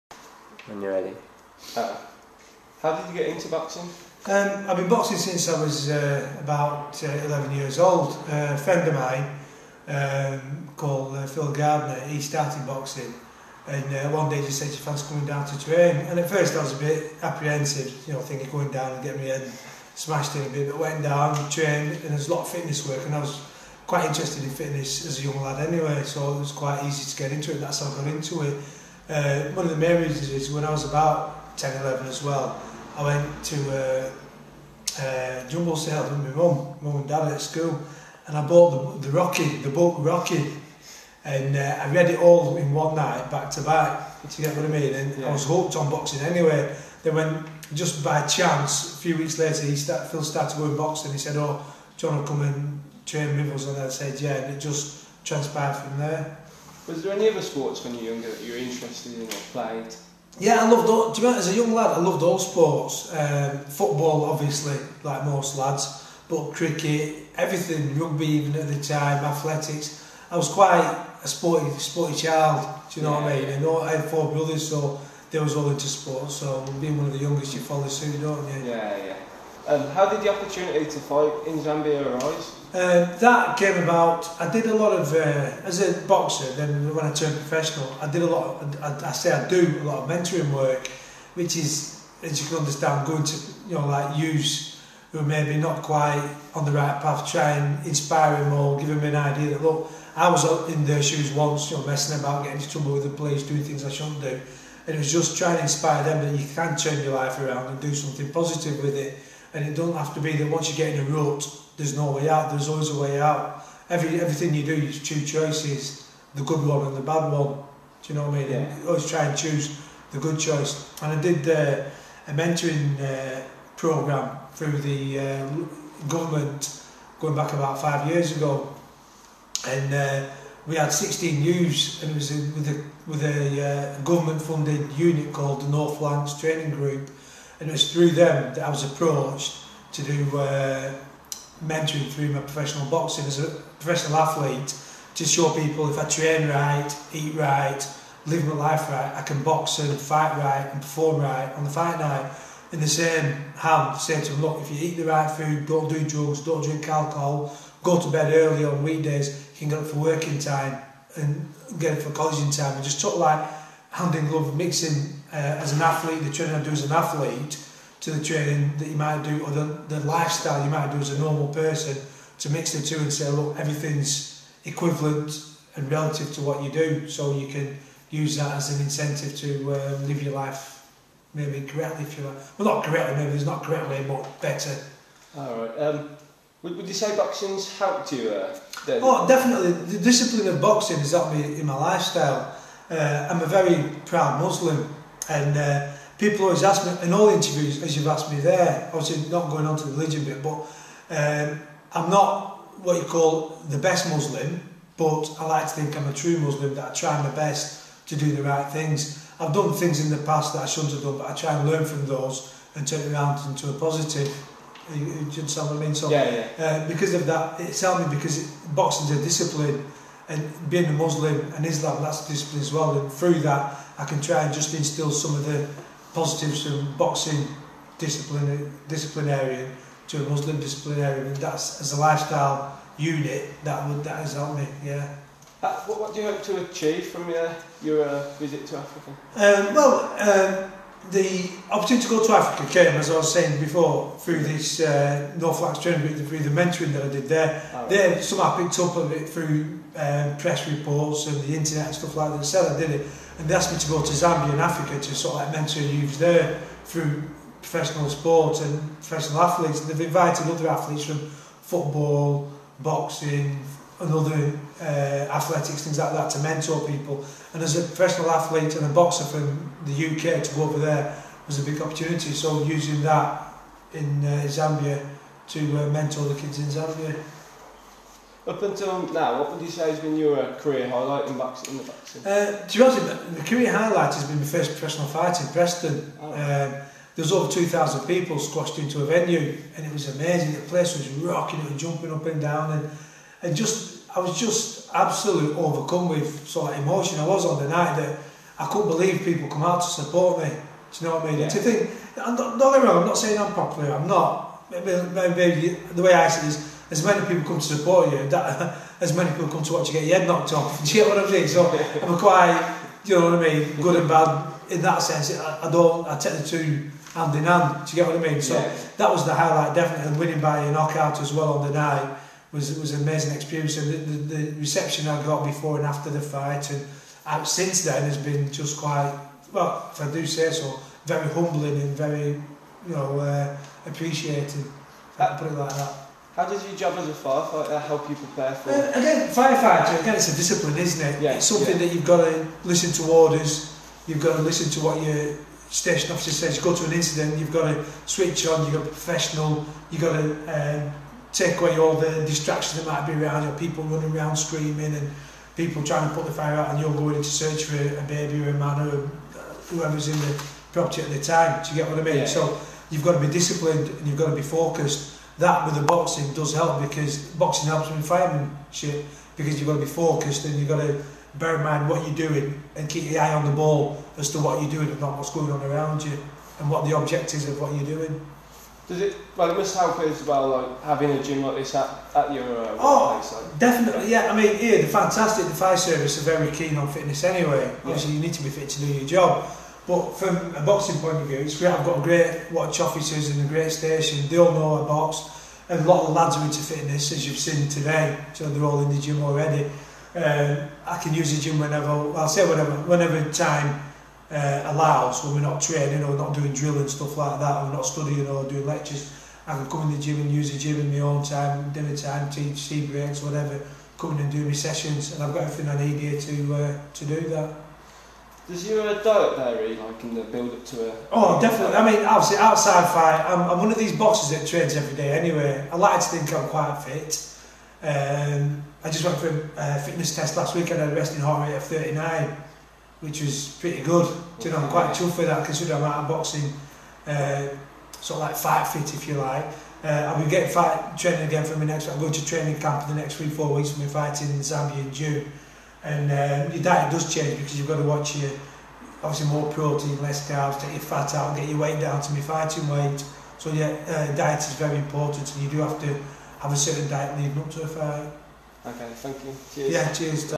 Suz Member Interview on Zambia Tour
Preston born Professional Boxer and local fireman Suz Member talks about his Boxing career and his surprise call up by the Zambian Government, to fight Live on national Television as well as mentor the youth over there.